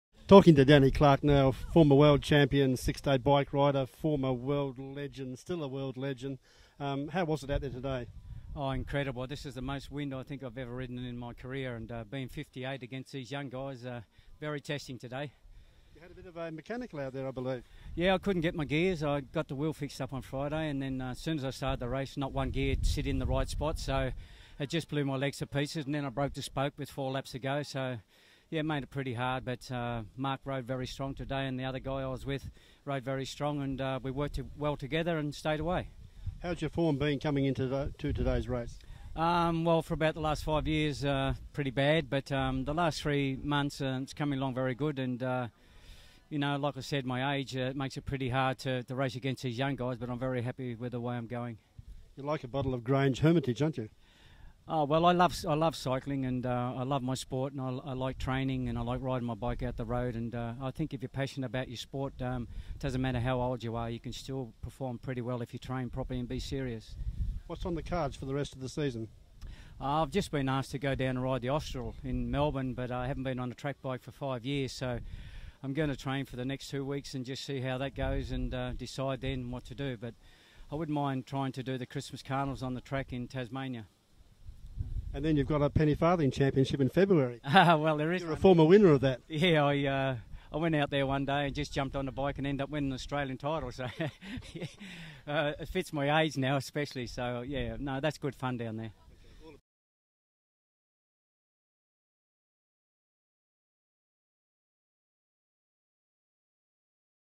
Danny Clark at the 2009 Darren Smith Classic
Cycling Legend Danny Clark speaks after getting 2nd at the Masters A / Darren Smith Classic.